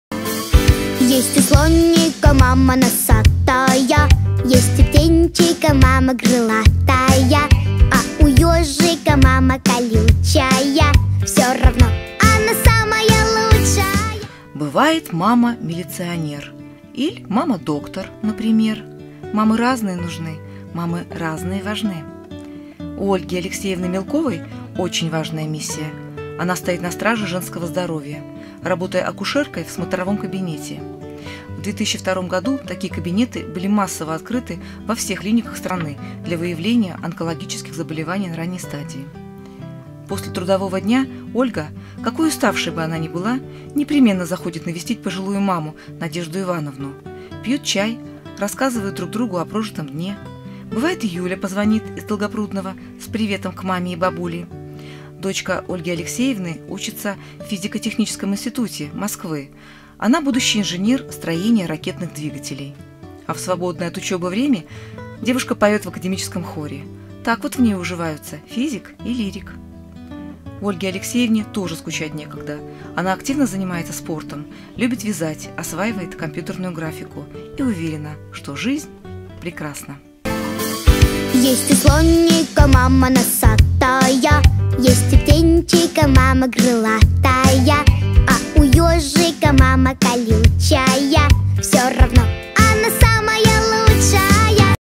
О наших мамах... Ольга Мелкова
"Мамы разные нужны, мамы разные важны" - писал Сергей Михалков, и был абсолютно прав. Накануне Дня матери мы начинаем цикл зарисовок о мамах Кумертау. Знакомьтесь, Ольга Мелкова с важной миссией - охраной женского здоровья в смотровом кабинете.